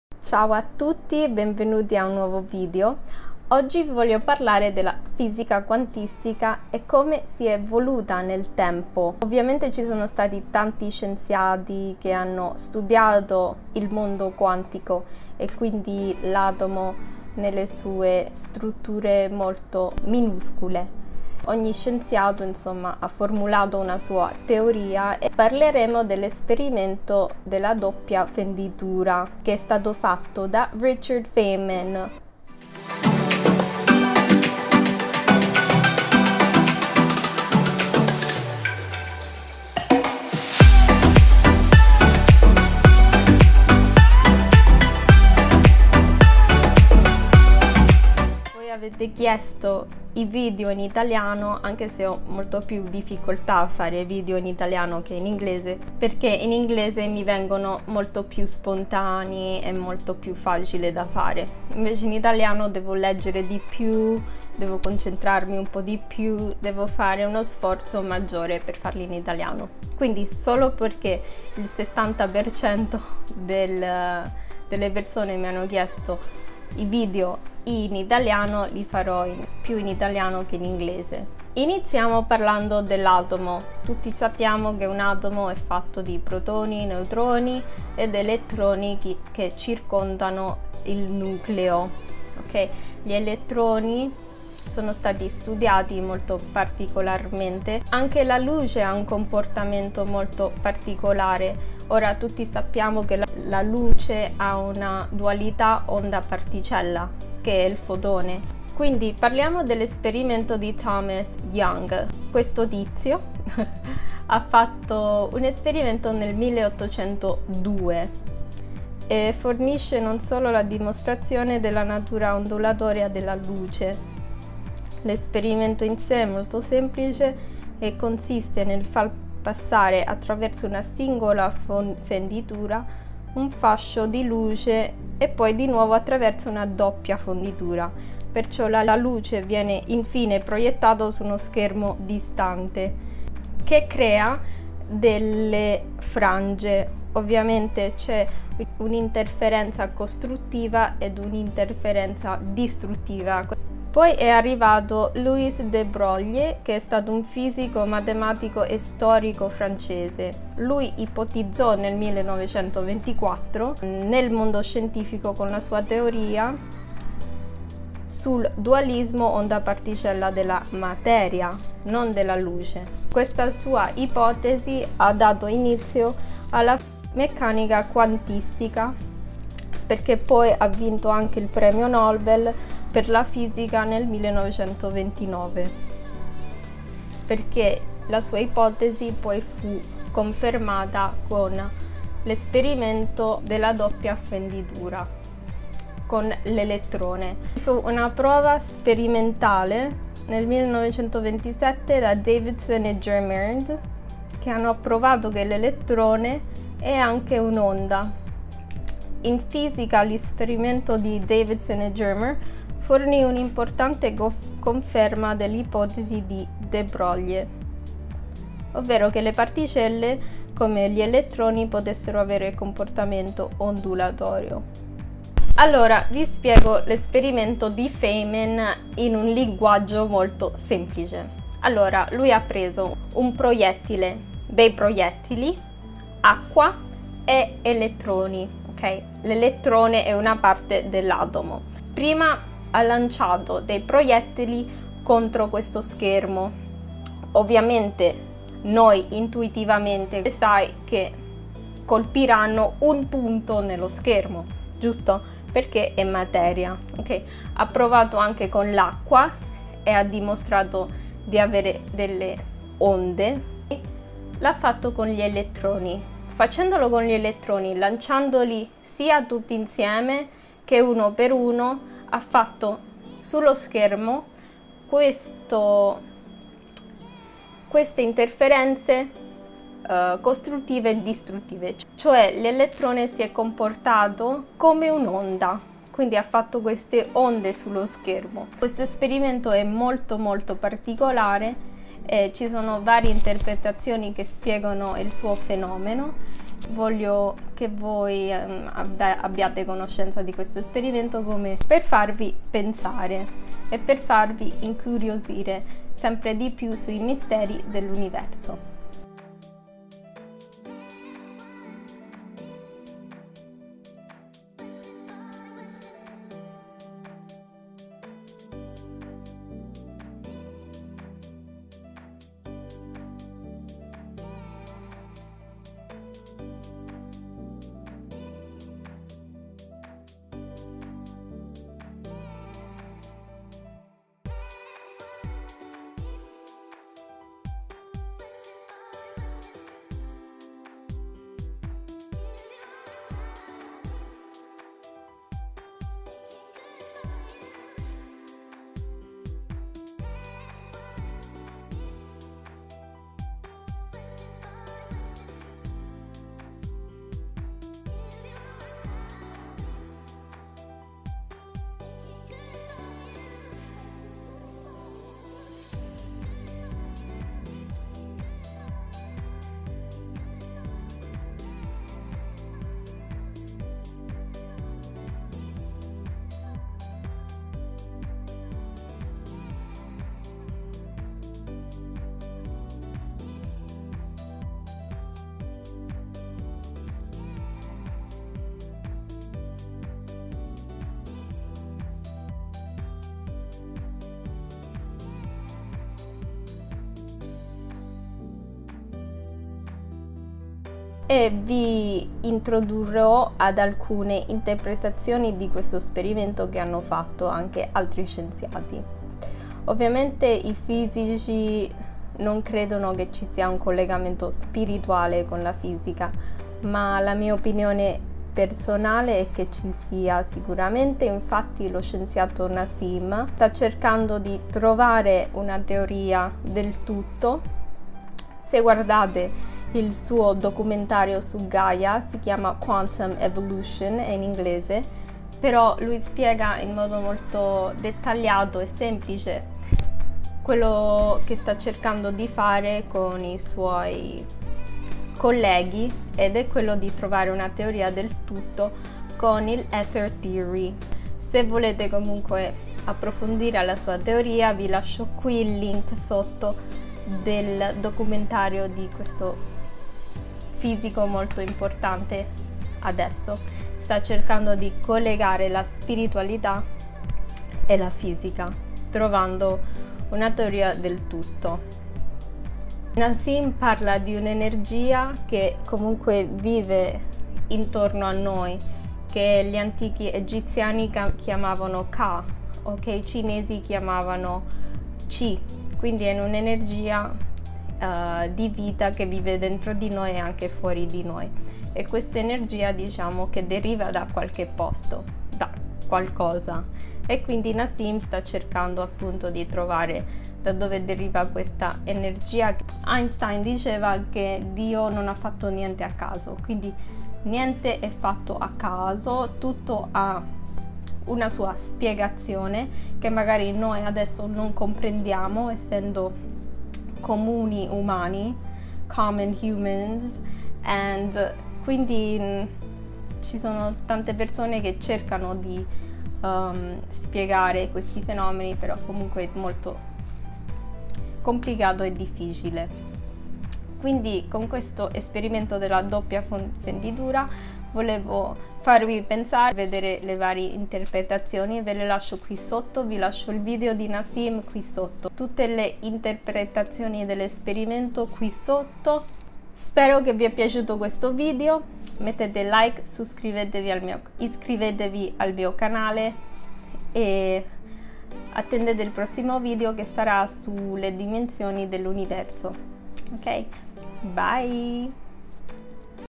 L'Esperimento della Doppia Fenditura e la Meccanica Quantistica | Giovanna Iaquinta
L'esperimento della doppia fenditura è una variante dell'esperimento di Young che permette di dimostrare il dualismo onda-particella della materia.
Richard Feynman era solito ripetere che questo esperimento rappresenta la chiave per la comprensione della meccanica quantistica, oltre a essere il fondamento della formulazione dell'integrale sui cammini da lui introdotta.

- Diverse Interpretazioni dell'esperimento
1.Copenhagen interpretation
2.Path-integral formulation
3.Relational interpretation
4.Many-worlds interpretation
5.De Broglie–Bohm theory